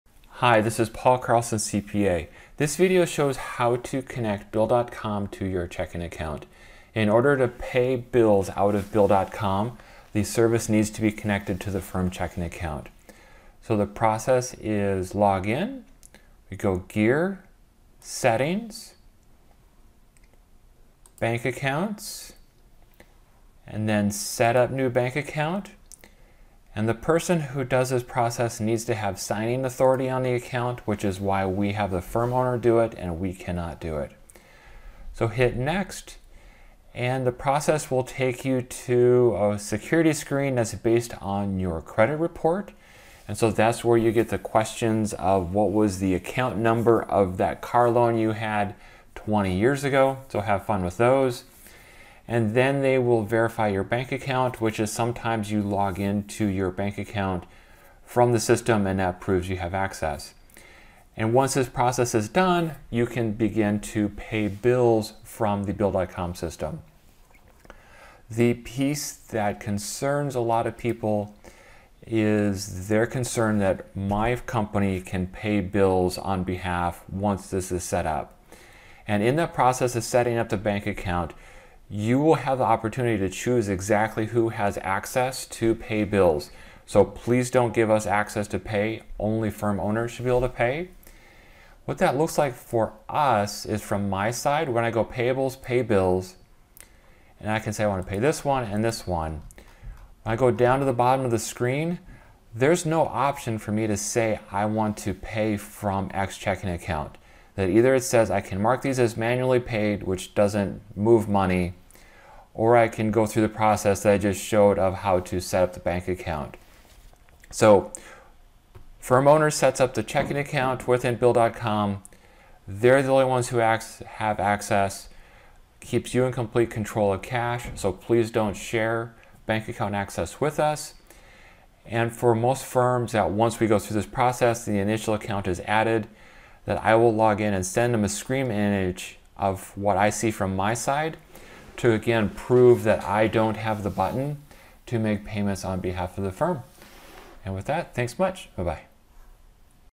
Bill.com - Add Checking Account
How to connect bill.com to the firm checking account.